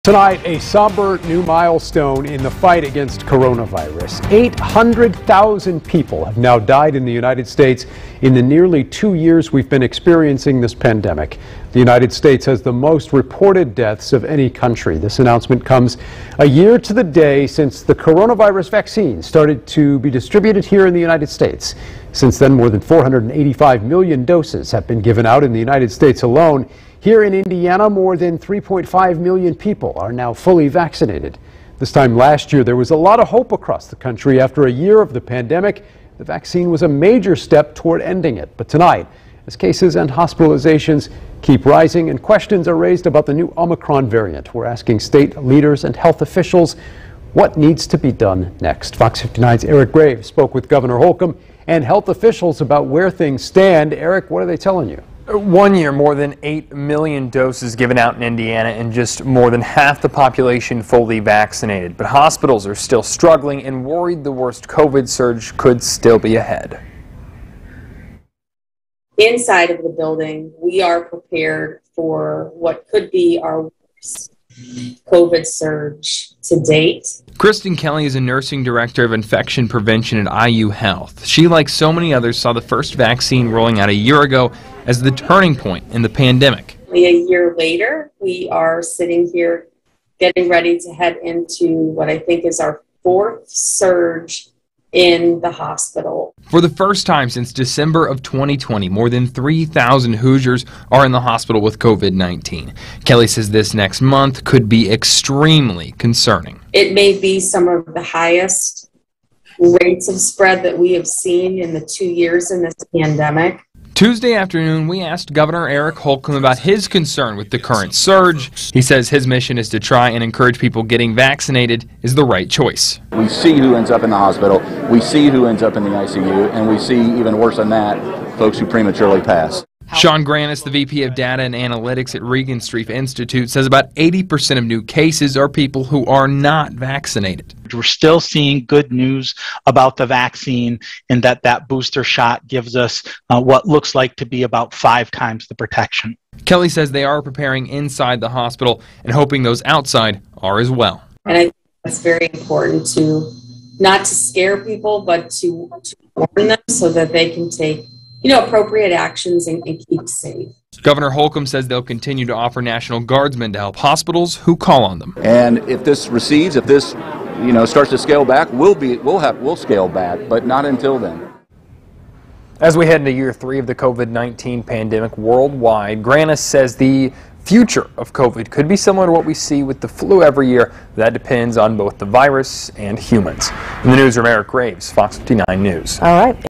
One year since first COVID-19 vaccine, Indiana sees cases and hospitalizations in fourth surge
Tuesday marks one year since the first COVID-19 vaccine was given out in the United States of America.